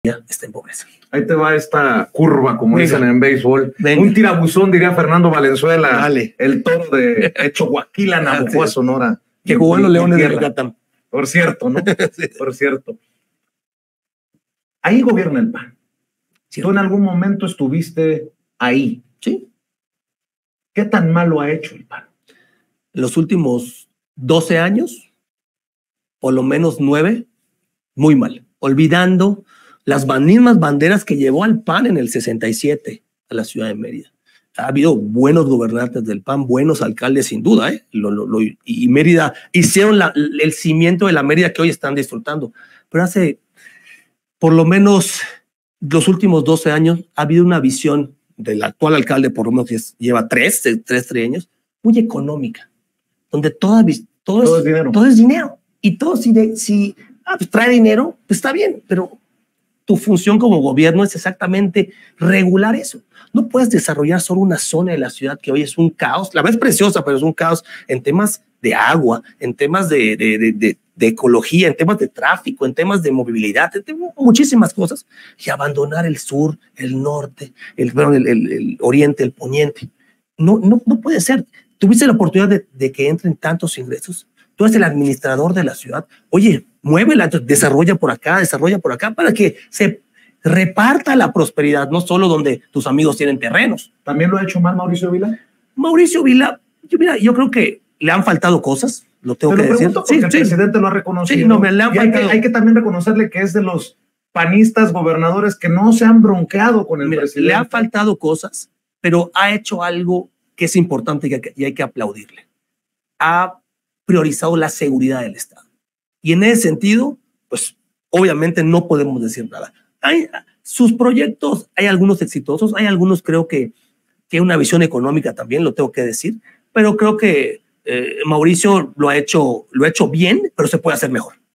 POR LO MENOS NUEVE AÑOS, EL PAN LO HA HECHO MUY MAL EN YUCATÁN, DICE RAÚL PAZ
El senador de Morena y aspirante a la gubernatura de Yucatán Raúl Paz Alonzo nos aseguró en exclusiva en Sin Censura como expanista que por lo menos nueve años, el PAN lo ha hecho muy mal en Yucatán, y en los últimos 12 años la visión es meramente económica. Nos comenta que para él a Mauricio Vila le han faltado cosas, pero que le reconoce priorizar la seguridad